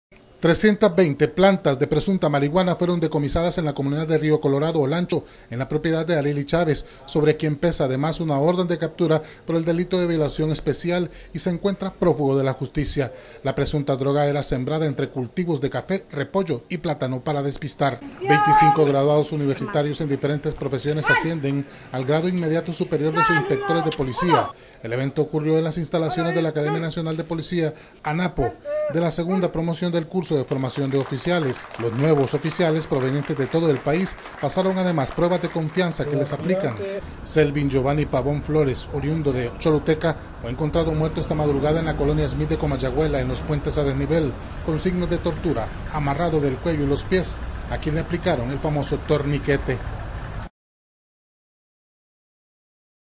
Sucesos Abriendobrecha Junio 08
320 plantas de presunta marihuana fueron decomisadas en la comunidad de rió colorado Olancho, 25 graduados universitarios en diferentes profesiones ascienden al grado inmediato superior de Subinspectores de Policía En las instalaciones de (ANAPO),  Selvin Geovanny pavón flores oriundo de Choluteca fue encontrado muerto esta madrugada en la colonia Smith de Comayagüela.